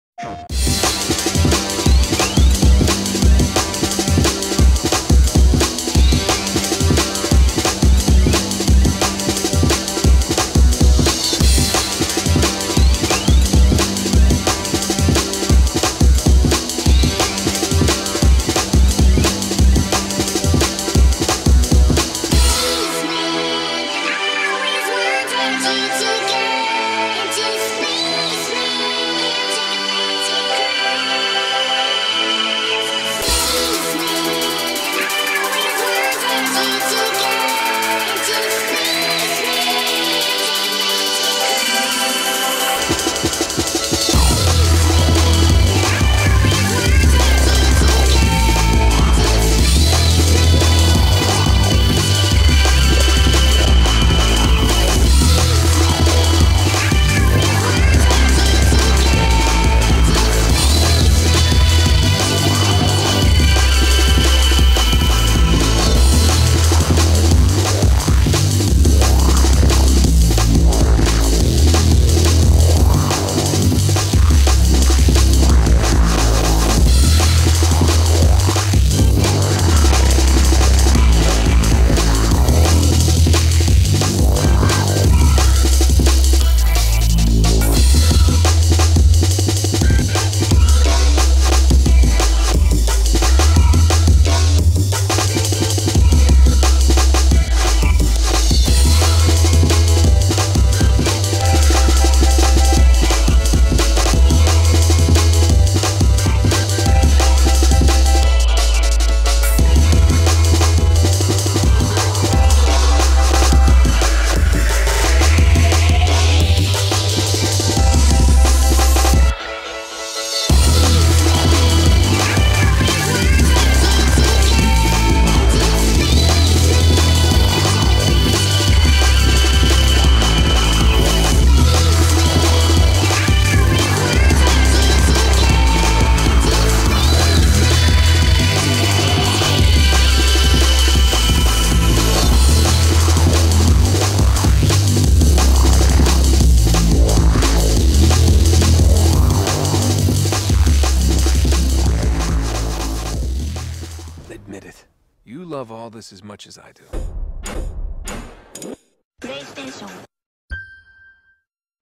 Playstation 3 Exclusives Tribute
Seeing as this generation of consoles is coming to an end, I decided to make a tribute for my favourite exclusives on the PS3 - My favourite console of this gen.